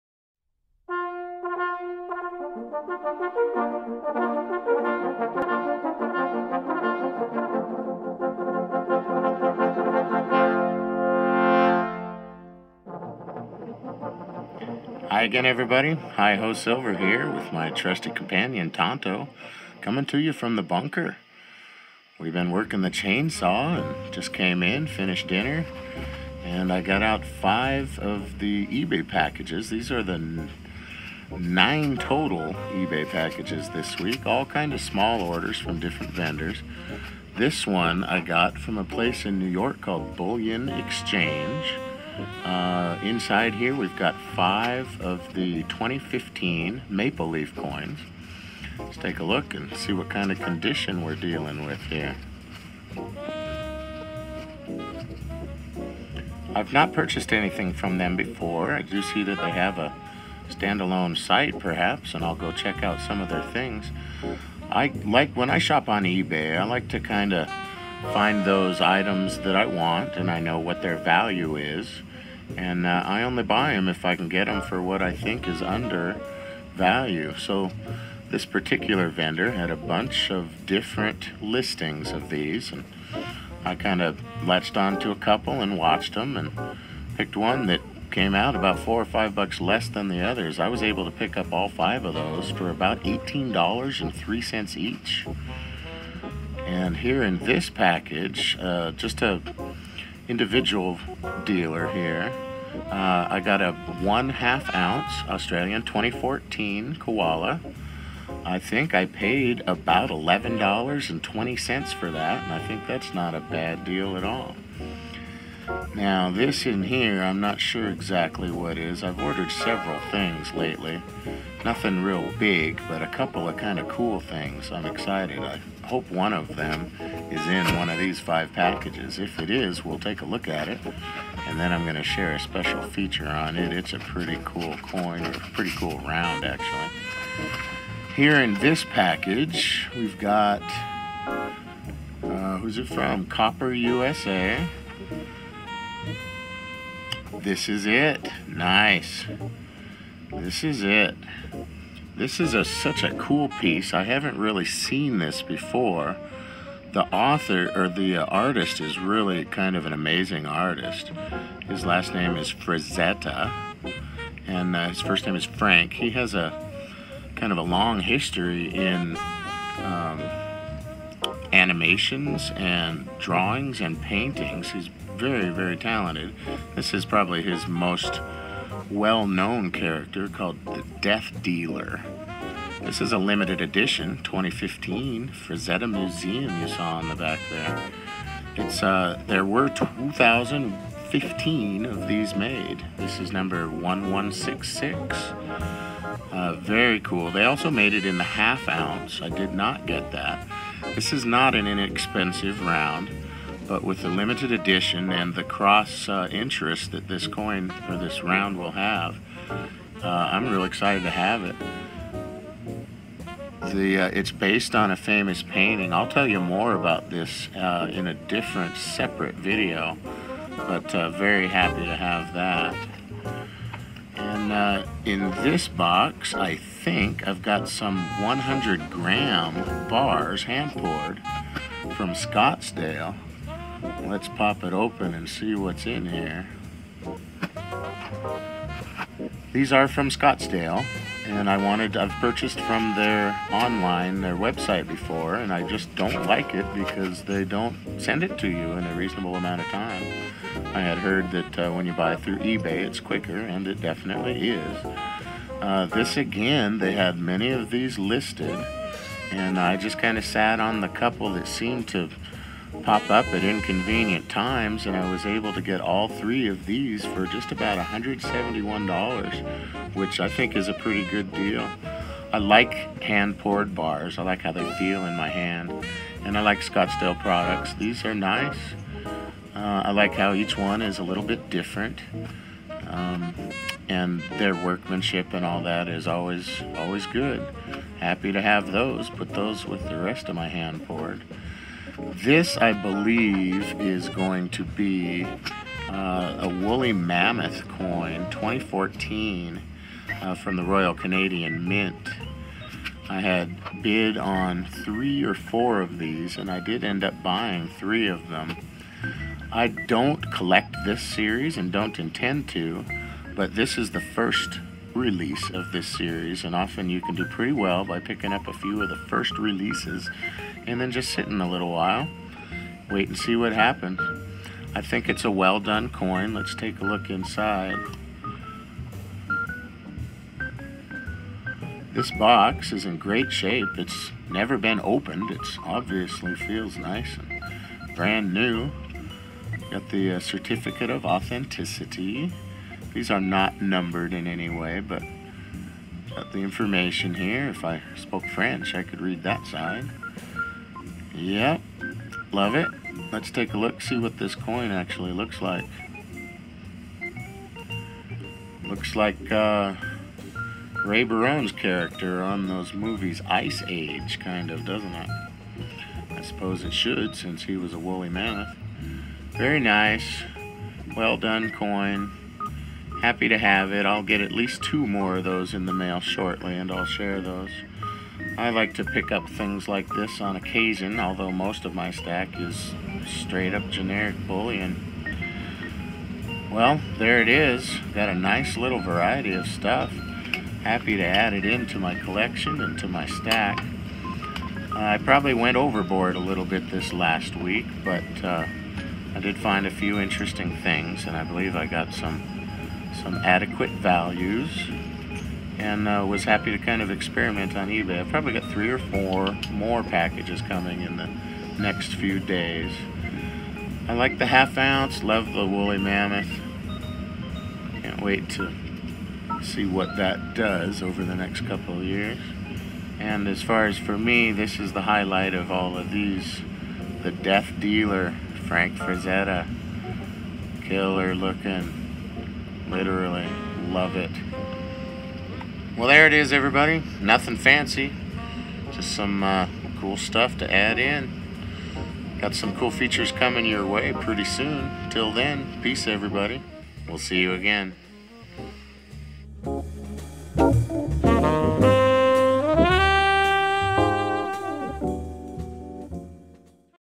Silver Bullion UnBoxing: 5 more eBay packages - Death Dealer and More - about 17 oz.
maple leafs - wooly mammoth - DEATH DEALER - scottsdale 100 gram bars - 1/2 oz koala - enjoy.  HiHo